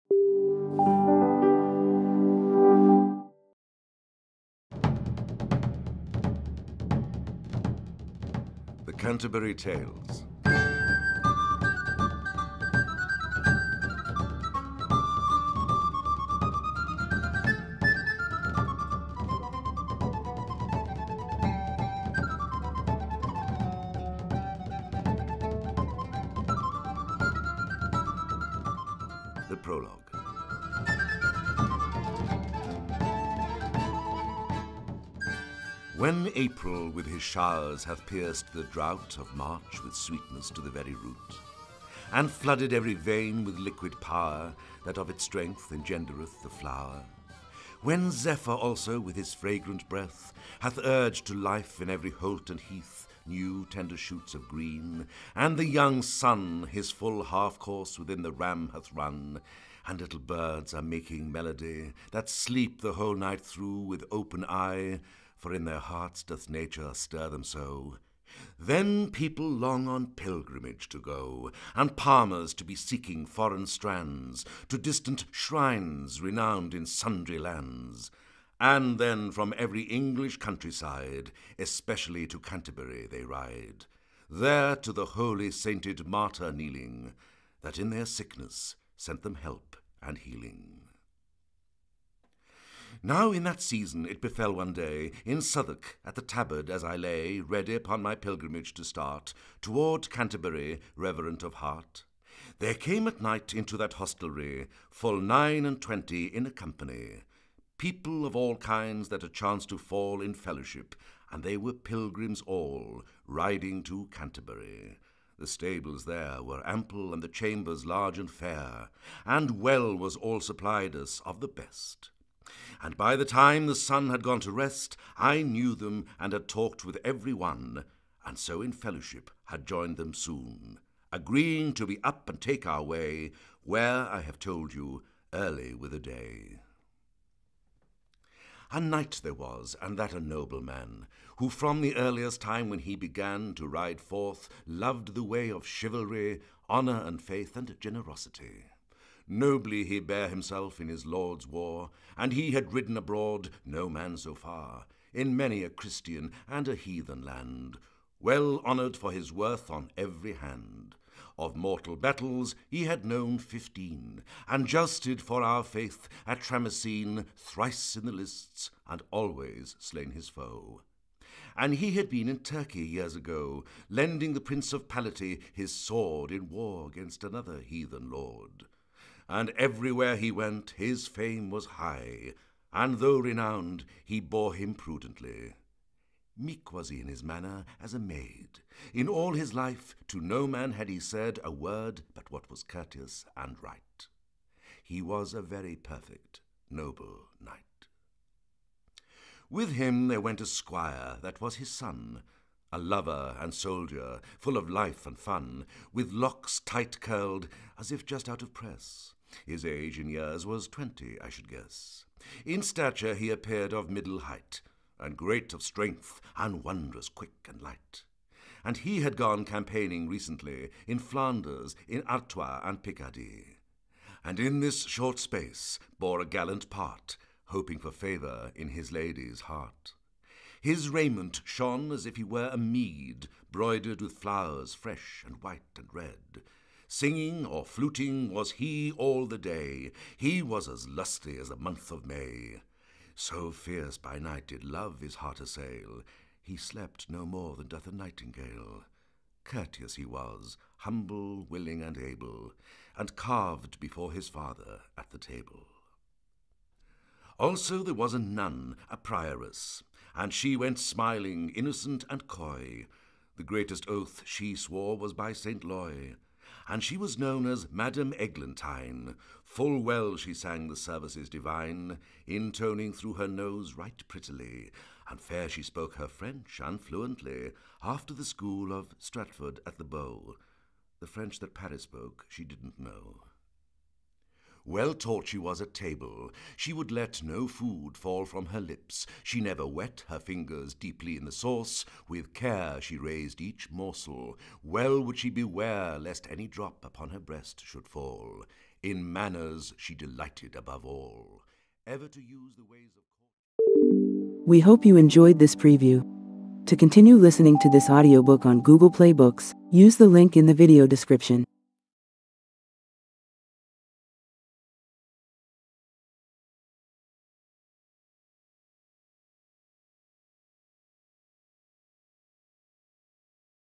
The Canterbury Tales by Geoffrey Chaucer · Audiobook preview
The Canterbury Tales
Authored by Geoffrey Chaucer
Narrated by Philip Madoc, Edward de Souza, Anthony Donovan, Clive Merrison, Clive Swift, Anton Lesser

0:00 Intro
0:03
5:03 Outro

BOOK DESCRIPTION

Chaucer’s greatest work, written towards the end of the fourteenth century, paints a brilliant picture of medieval life, society, and values. The stories range from the romantic, courtly idealism of The Knight’s Tale to the joyous bawdy of the Miller’s; all are told with a freshness and vigour in this modern verse translation that make them a delight to hear.

AUDIOBOOK DETAILS

Language: English
Publisher: Naxos AudioBooks
Published on: October 1, 2004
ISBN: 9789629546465
Duration: 3 hr, 22 min
Genres: Poetry / Medieval